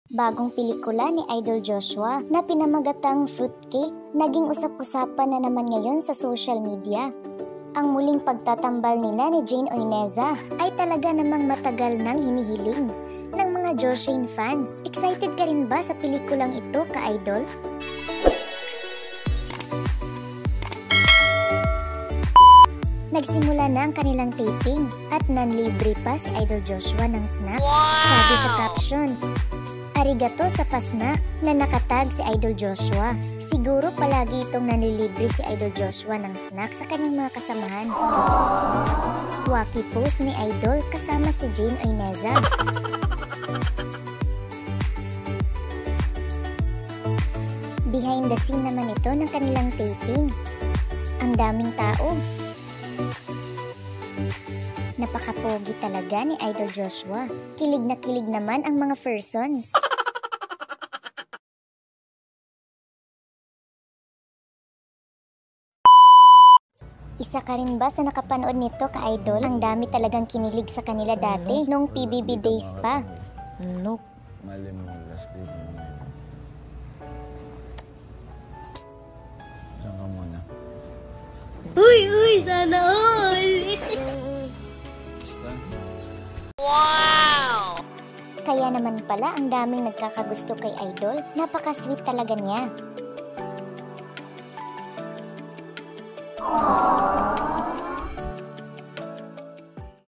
Joshua Garcia at Jane Oineza, tuloy tuloy ang Taping sa kanilang Film Project, behind the scene!🥰🥰🥰
Joshua Espineli Garcia (born October 7, 1997), known professionally as Joshua Garcia, is a Filipino actor, dancer, and commercial model.
Elizabeth Jane Urbano Oineza (born July 22, 1996), better known as Jane Oineza, is a Filipino actress, commercial model and singer.